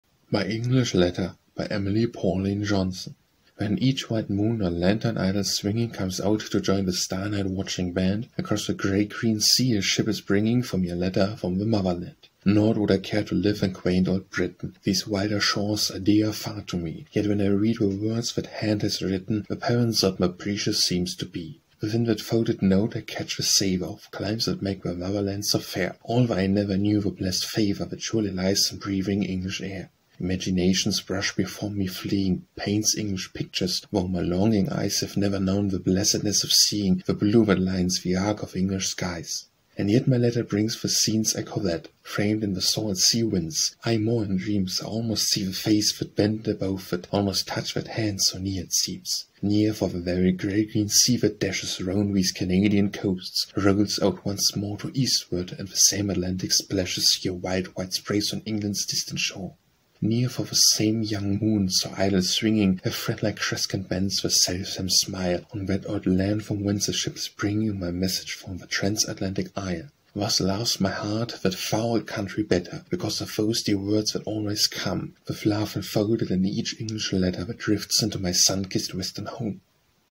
My English Letter - Emily Pauline Johnson
My English Letter
Emily Pauline Johnson

    When each white moon, her lantern idly swinging,
        Comes out to join the star night-watching band,
    Across the grey-green sea, a ship is bringing
        For me a letter, from the Motherland.

    Naught would I care to live in quaint old Britain,
        These wilder shores are dearer far to me,
    Yet when I read the words that hand has written,
        The parent sod more precious seems to be.

    Within that folded note I catch the savour
        Of climes that make the Motherland so fair,
    Although I never knew the blessed favour
        That surely lies in breathing English air.

    Imagination's brush before me fleeing,
        Paints English pictures, though my longing eyes
    Have never known the blessedness of seeing
        The blue that lines the arch of English skies.

    And yet my letter brings the scenes I covet,
        Framed in the salt sea winds, aye more in dreams
    I almost see the face that bent above it,
        I almost touch that hand, so near it seems.

    Near, for the very grey-green sea that dashes
        'Round these Canadian coasts, rolls out once more
    To Eastward, and the same Atlantic splashes
        Her wild white spray on England's distant shore.

    Near, for the same young moon so idly swinging
        Her threadlike crescent bends the selfsame smile
    On that old land from whence a ship is bringing
        My message from the transatlantic Isle.

    Thus loves my heart that far old country better,
        Because of those dear words that always come,
    With love enfolded in each English letter
        That drifts into my sun-kissed Western home.